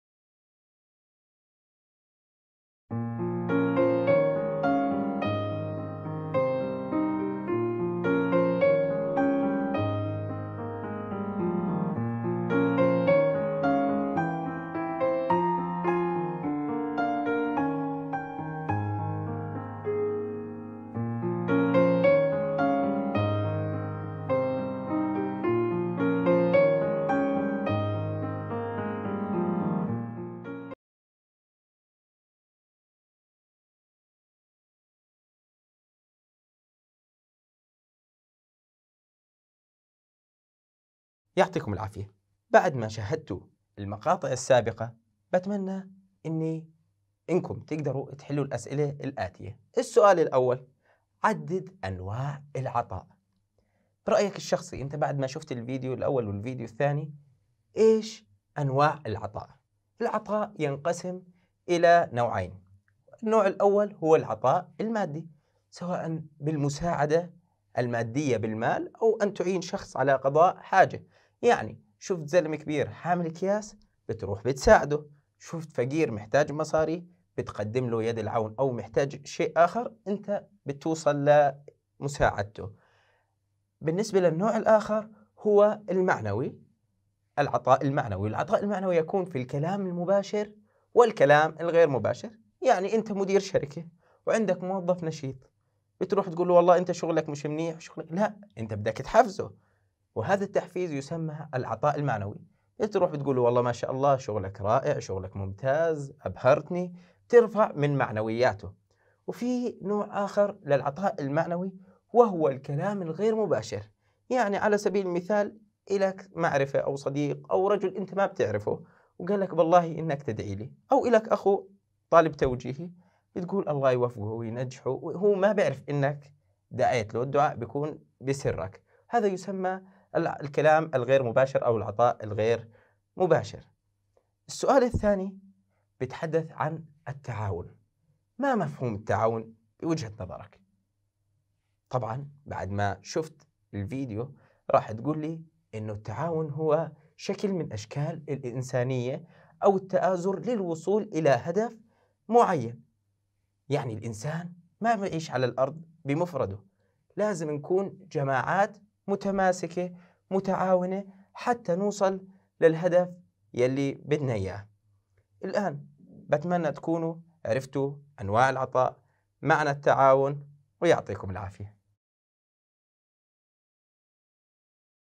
عربي صف سابع - أنواع العطاء
عربي صف سابع
 الحرية والتسامح - التحدث - أنواع العطاء
(الفصل الاول الوحدة الثالثة)

تعلم اللغة العربية مع الأستاذ عادل عودة على منصة أبواب